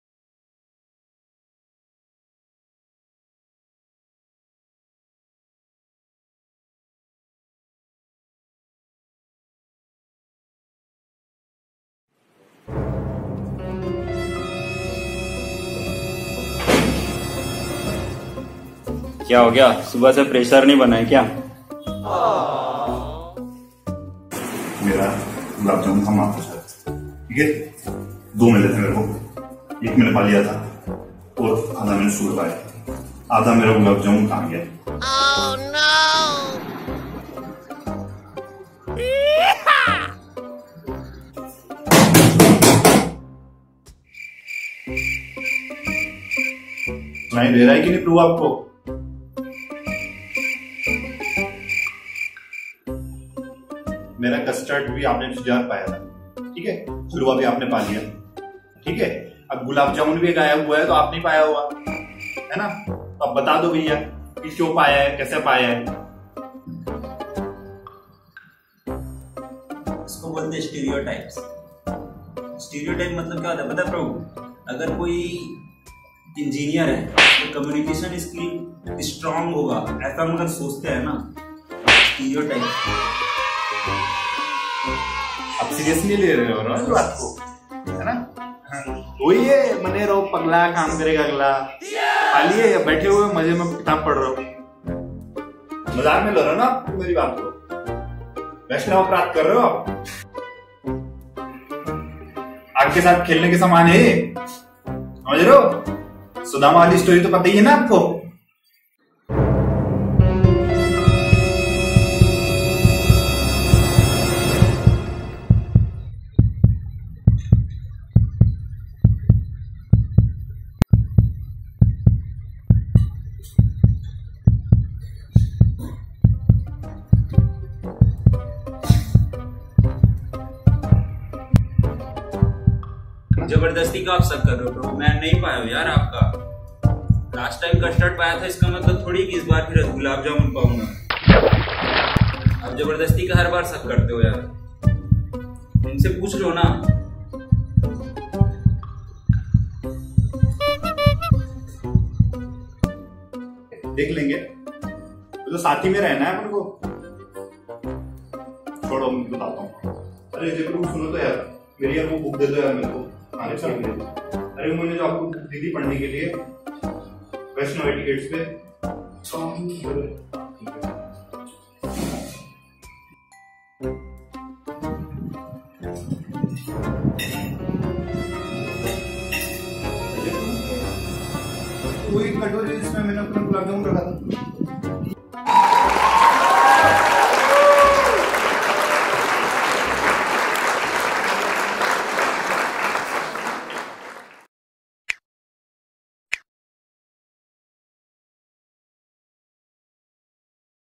महाप्रसाद || Funny Short Film
story of mahaprasad in BACE with neophyte devotees